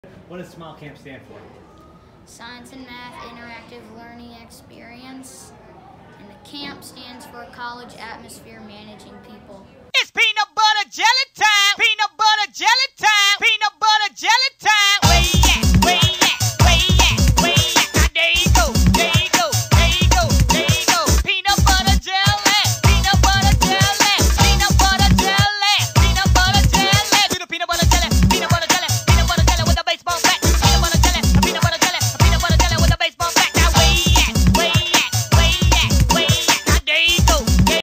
SMILECAMP.org located near NC State
Located at 6301 Hillsborough St, Raleigh, NC 27606 SMILE Camp is just down the street from the world famous NC State campus. NC State is known for it's break through research in area of Science and Mechanical Engineering. Roger, a NC State alumni and his wife Karen decided that they wanted to help academically gifted students to grow in their love for Science and Math.